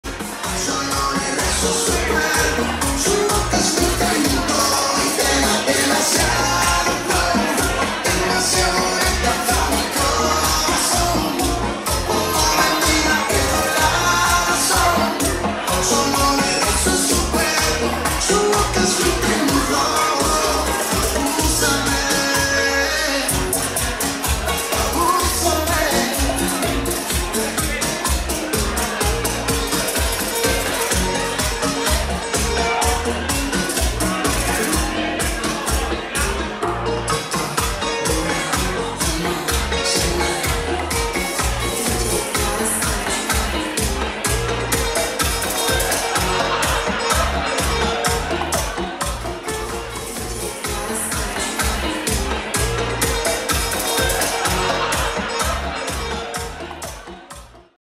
Mauricio Reyes & Elizabeth Dickinson REBEL SBK NOCHES 2023
Social 💯
REBEL SBK NOCHES 2023
Are you ready for the event on April 26?
Dancers: @mauricioreyesofficial & @_elizabethdickinson

Videographer: @socialdancetv_uk

Organizer: Andreas RU Salsa Rebel

📽️ You can follow our Youtube channel for 4K 16x9 full  videos in original format. Thank you.

Youtube: Social Dance TV UK